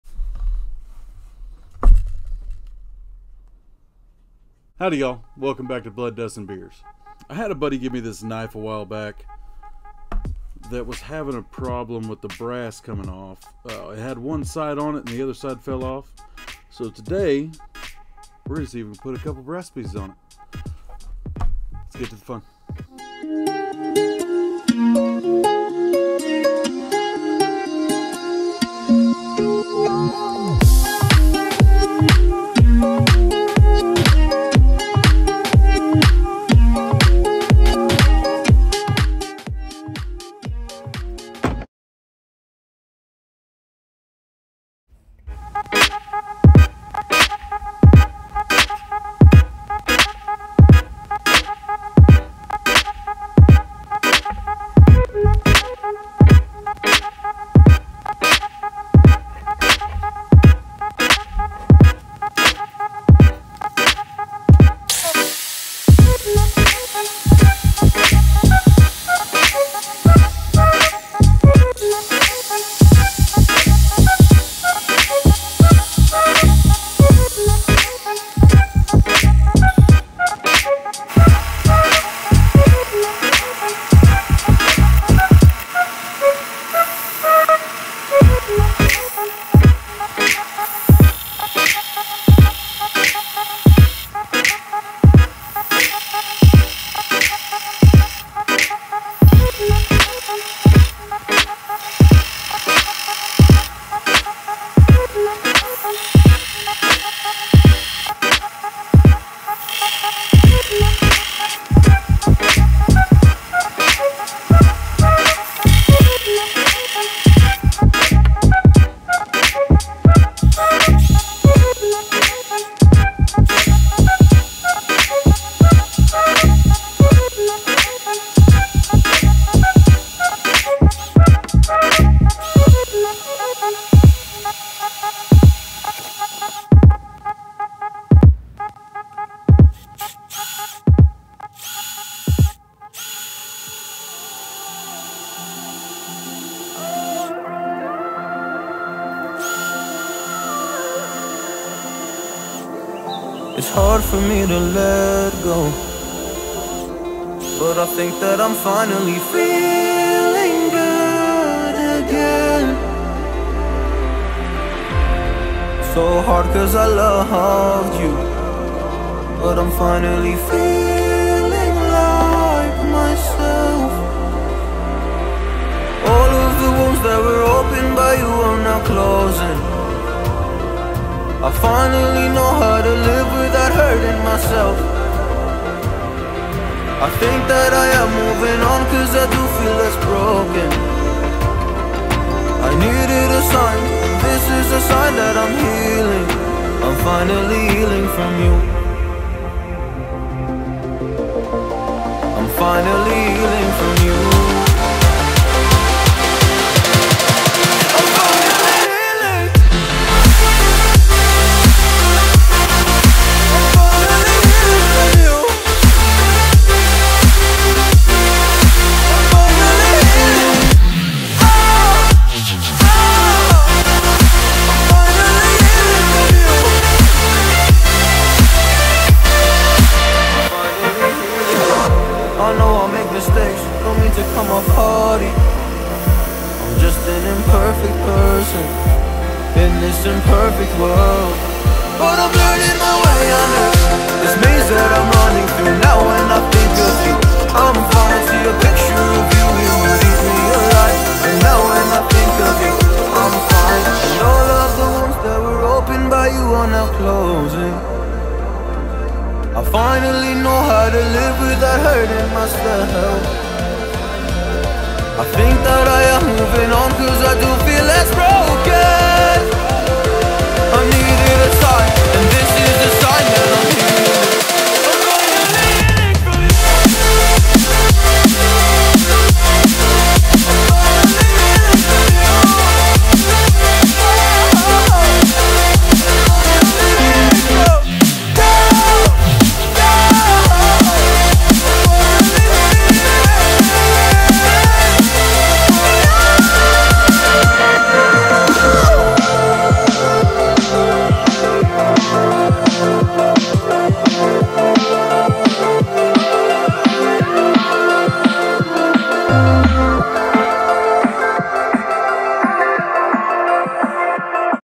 How to make a knife bolster | DIY knife bolster(for beginners)
How to make a knife bolster. DIY. Fixing a broken bolster.